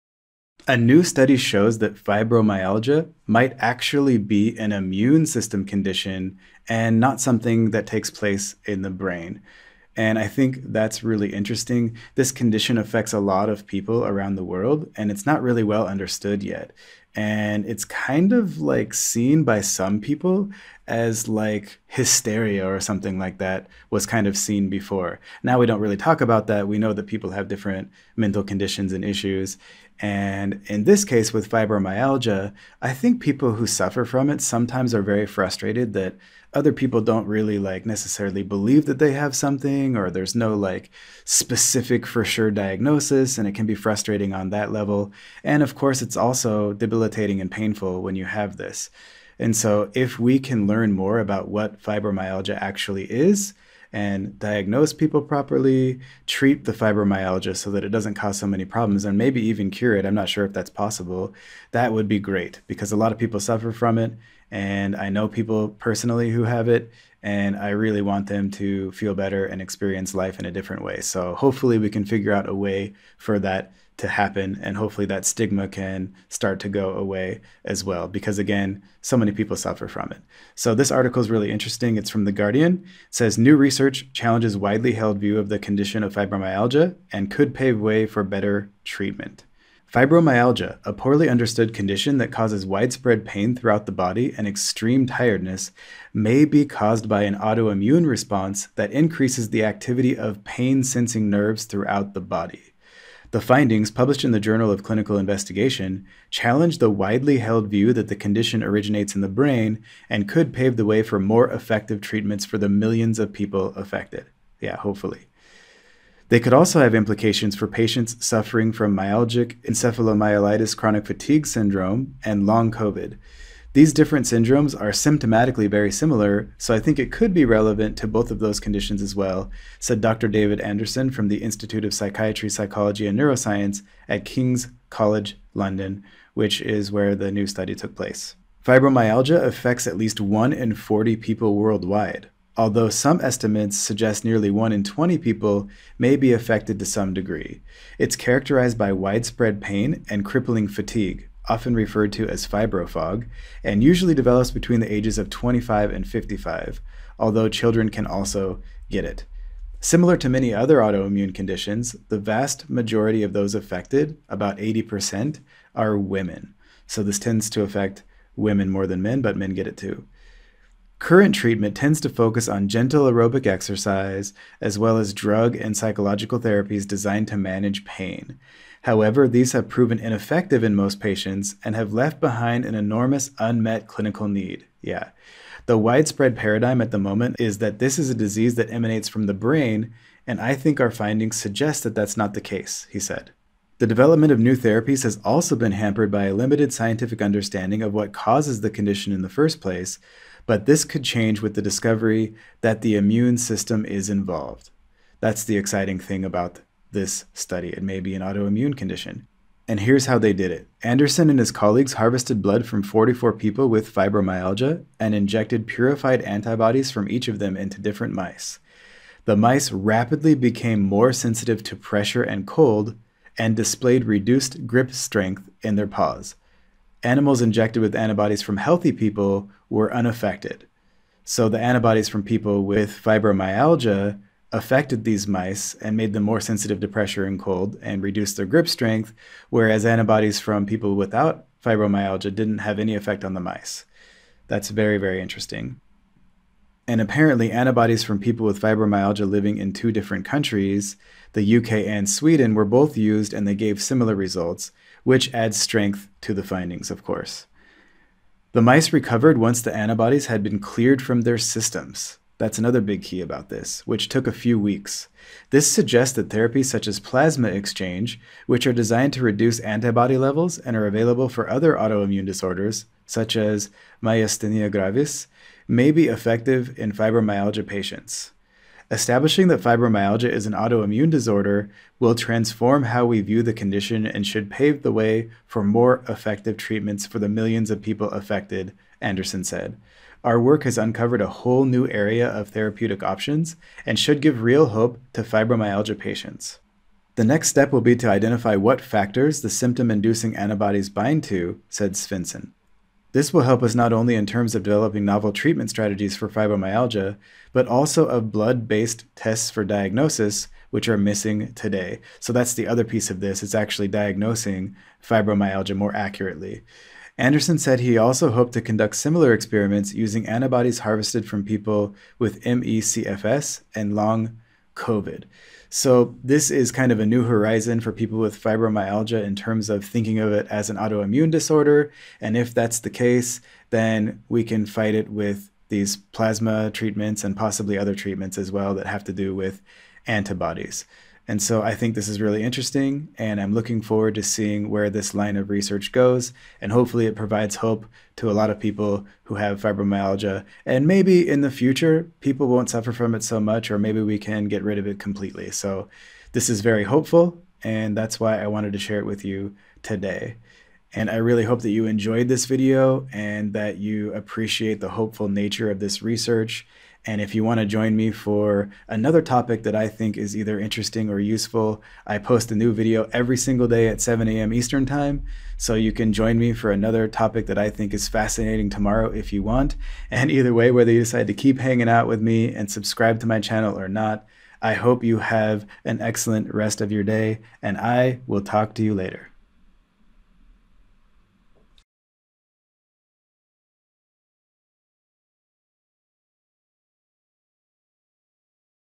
Fibromyalgia May Be an Autoimmune Disorder...
A new study shows that fibromyalgia may in fact be an autoimmune disorder, not a neurological condition or other type of condition. If fibromyalgia is an autoimmune disorder, new treatments for this common illness can be developed in the near future that are much more effective.

New video every M-F at 7 AM Eastern...